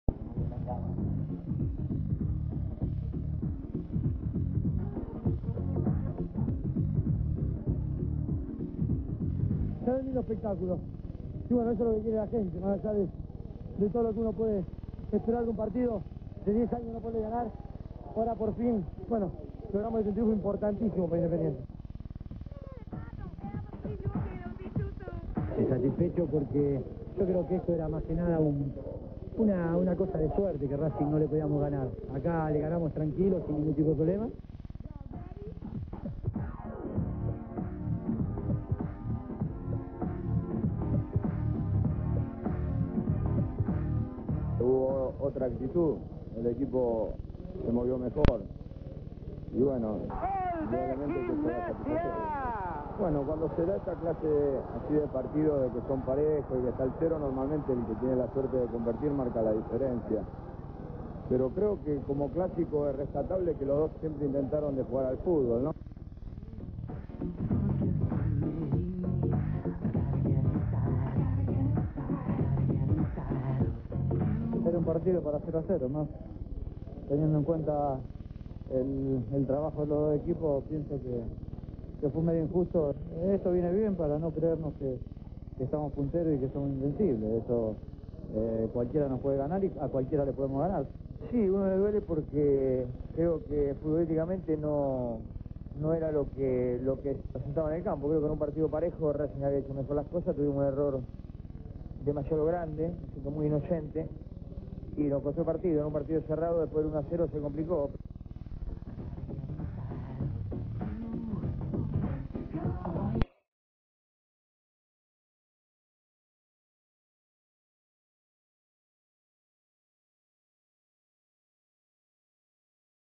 Racing Club vs Independiente - Luis Islas Miguel Brindisi Jorge Borelli 1994 V-07662 DiFilm
Fecha: 30/1/94 30 de enero de 1994
Código: V-07662